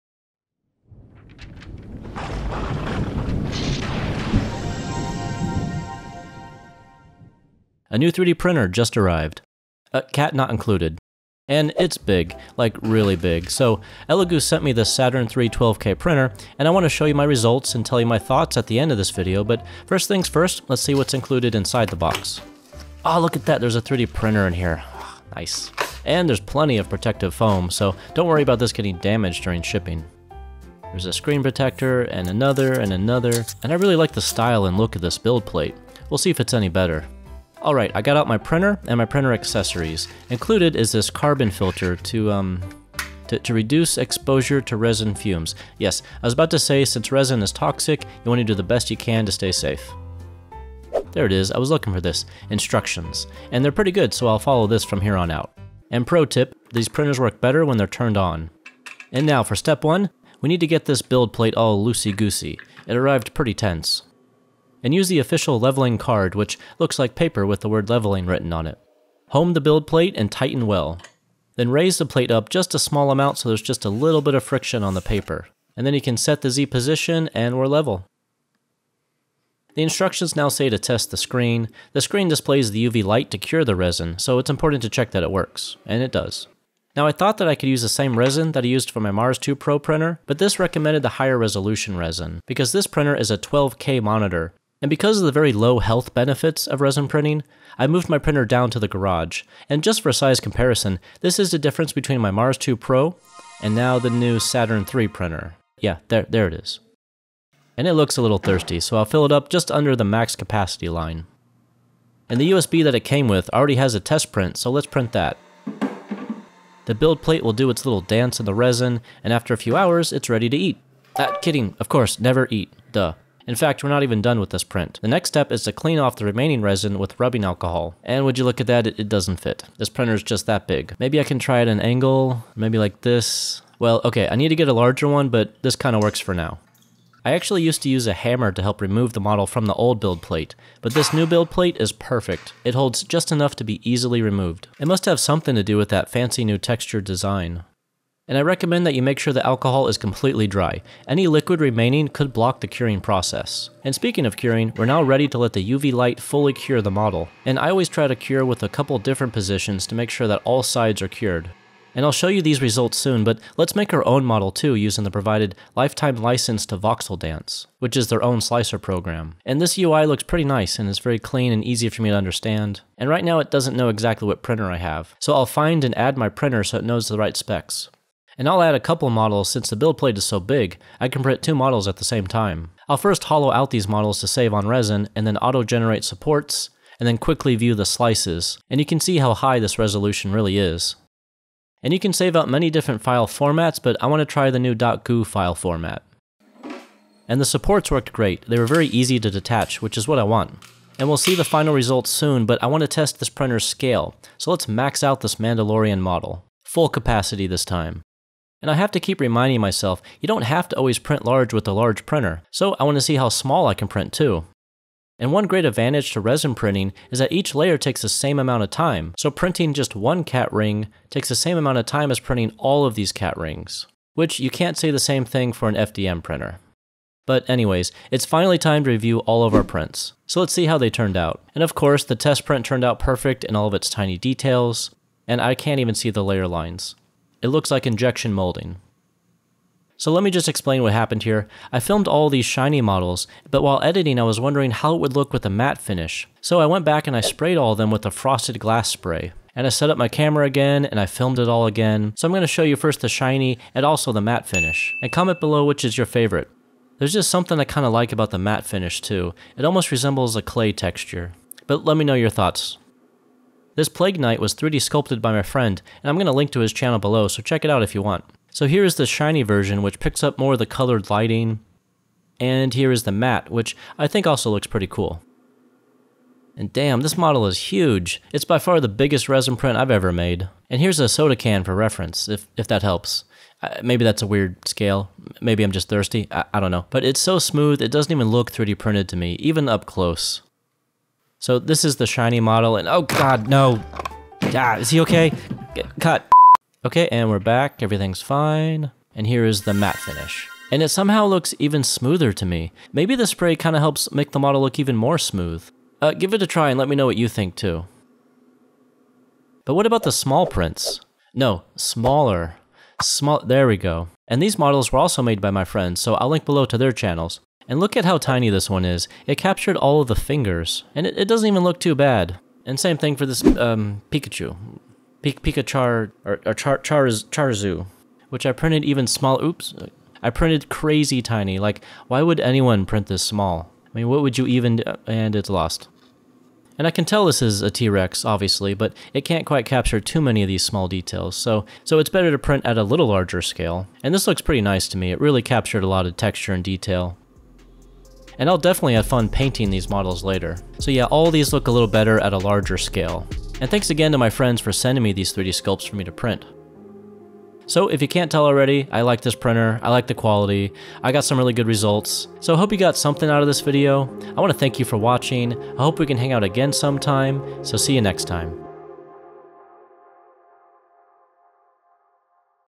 Huge 12K Resin Printer and My Experience | The Saturn 3
I wanted to share my thoughts, experience, and final print results with you from this Saturn 3 12K resin printer.

-Sage